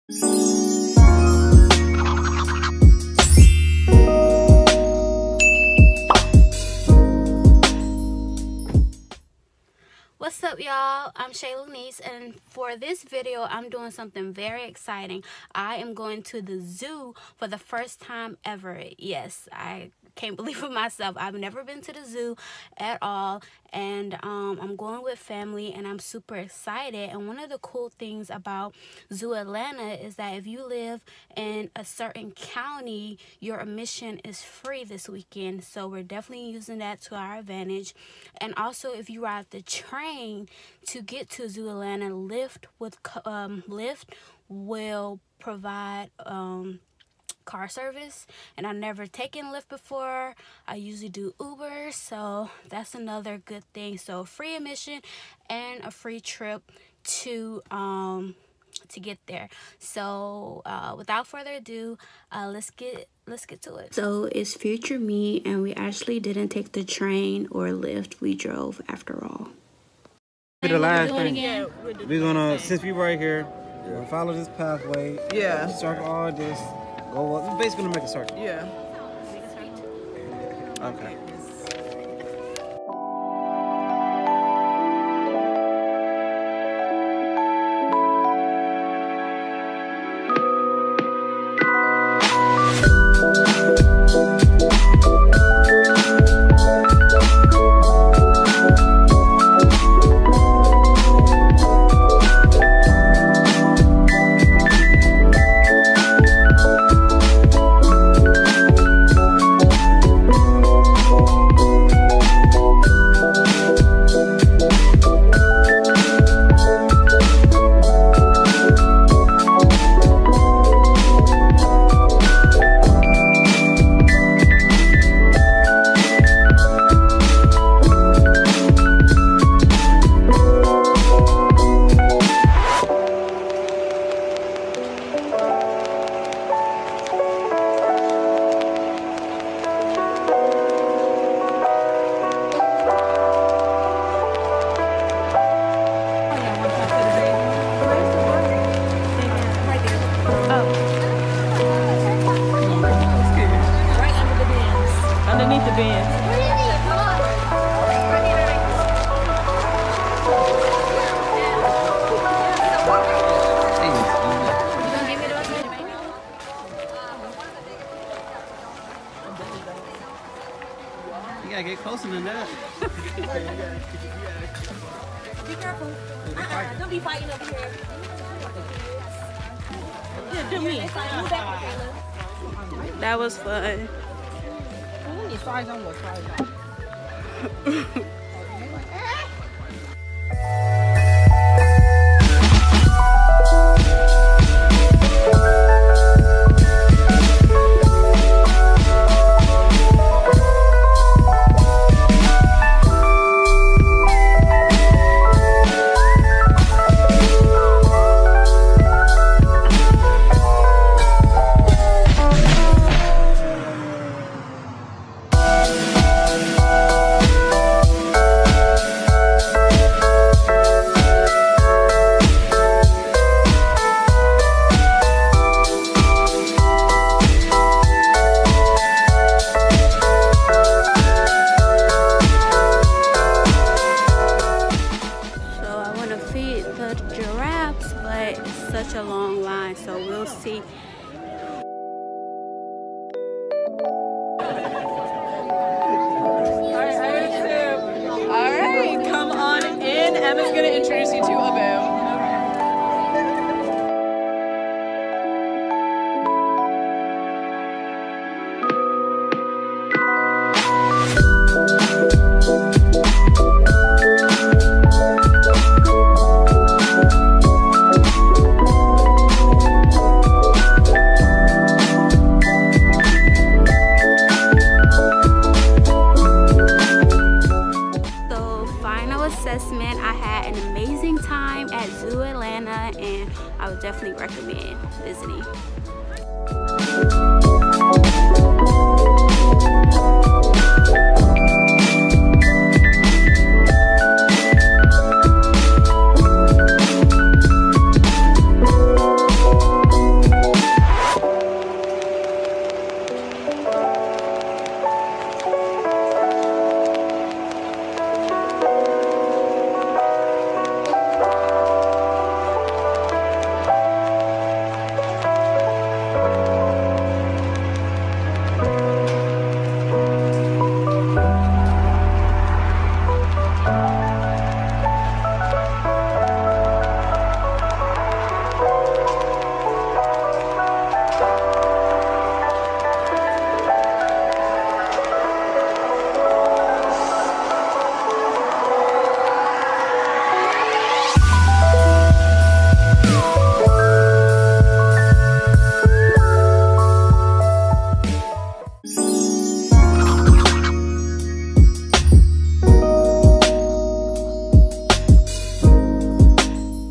FIRST TIME VISITING ZOO ATLANTA | VLOG
Hello everyone! For this vlog, I’ll be sharing my experience visiting Zoo Atlanta for the first time ever!

~SOCIAL MEDIA~
Instagram | Facebook | Twitter : @ShayLanise